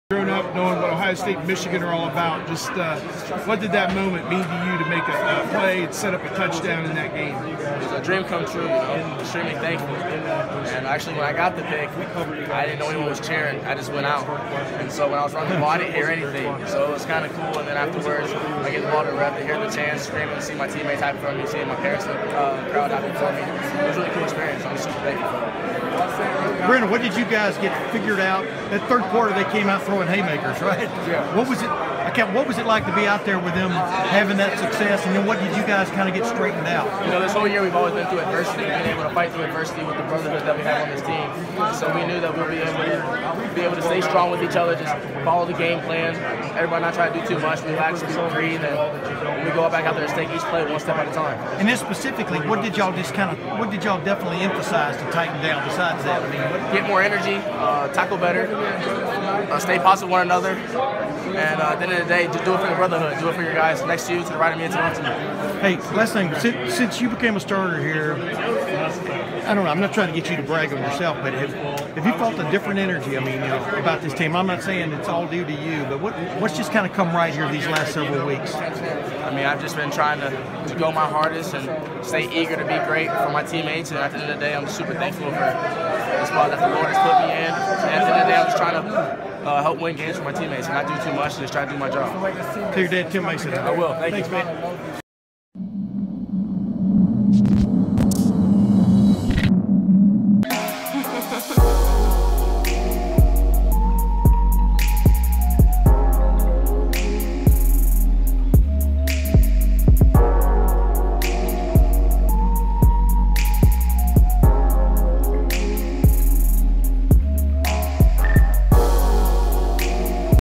Ohio State safety Brendon White on Buckeyes Fighting Through Adversity
Watch as Ohio State safety Brendon White speaks on the Buckeyes Fighting Through Adversity